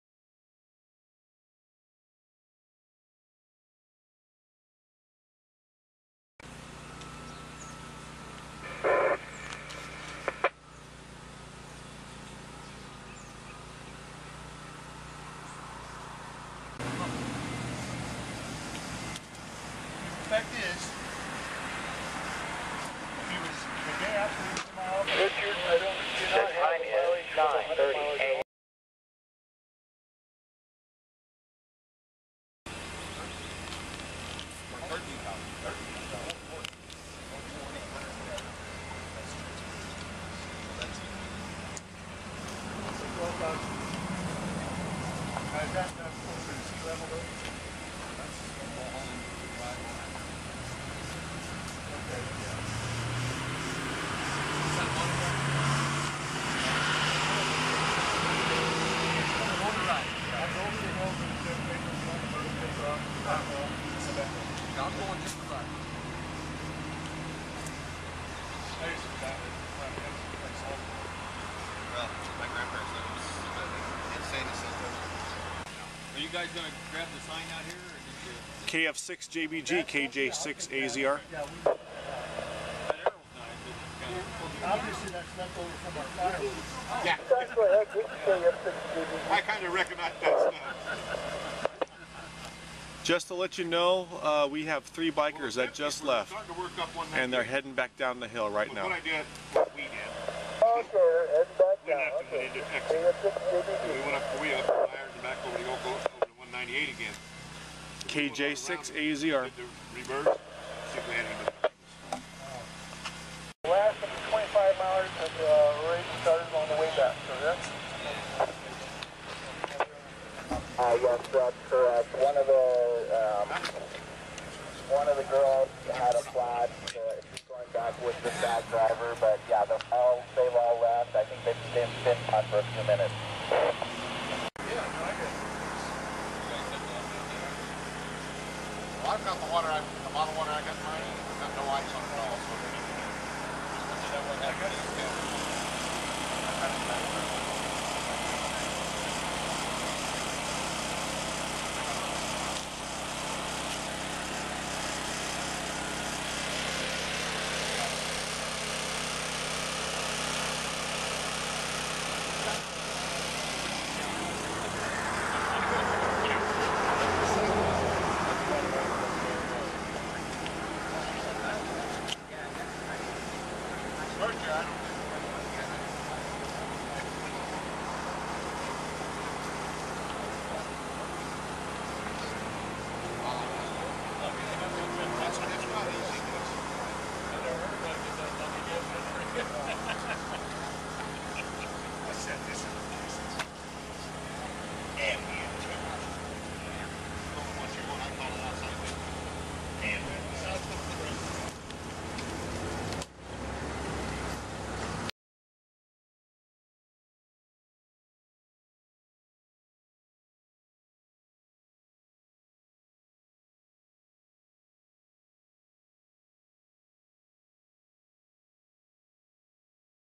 VALLEY SPRING BICYCLE RIDE 2013 REST STOP
This is a rest stop for a bike ride.  Ham radio operators are located at the larger stops and I'm at the halfway point.